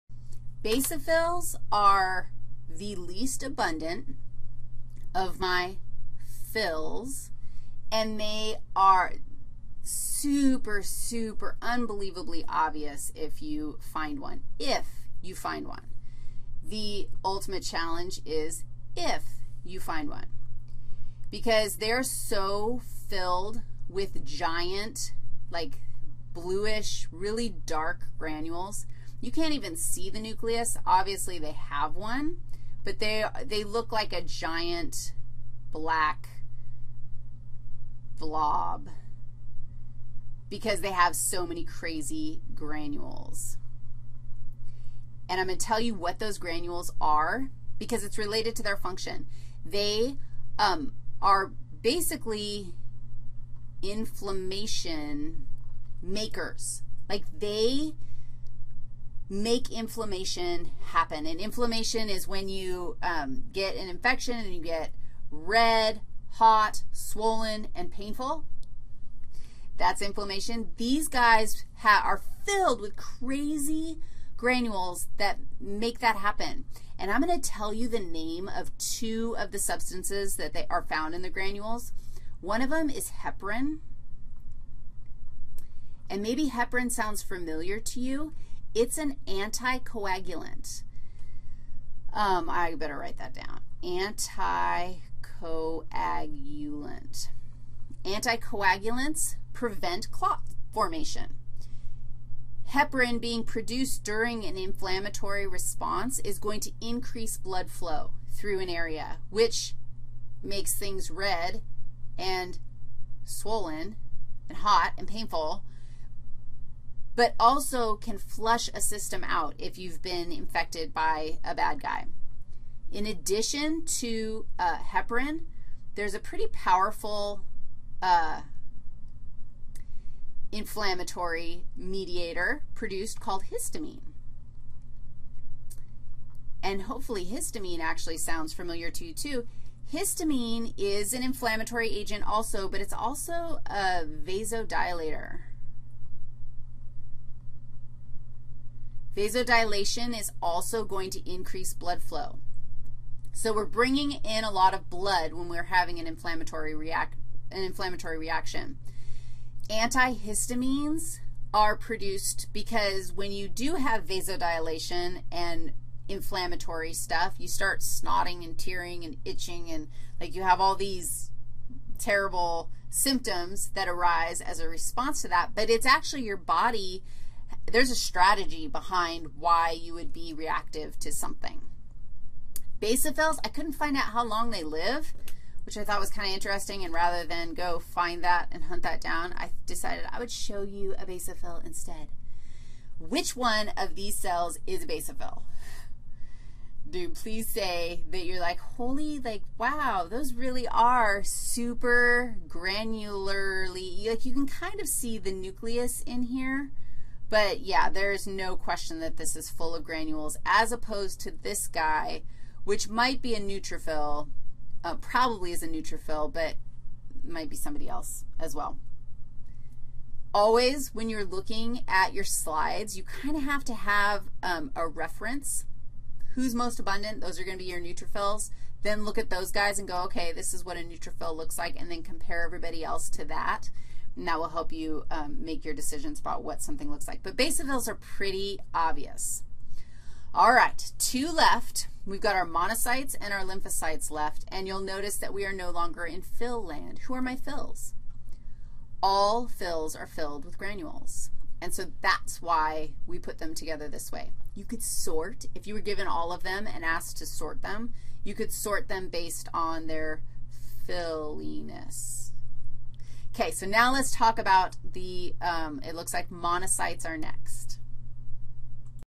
Blood 7- Basophils
Part 7 of 9 in a lecture on BLOOD in a flipped Human Anatomy course taught by Wendy Riggs. CC-BY. Watch the whole lecture (all 9 videos) by going to the PLAYLIST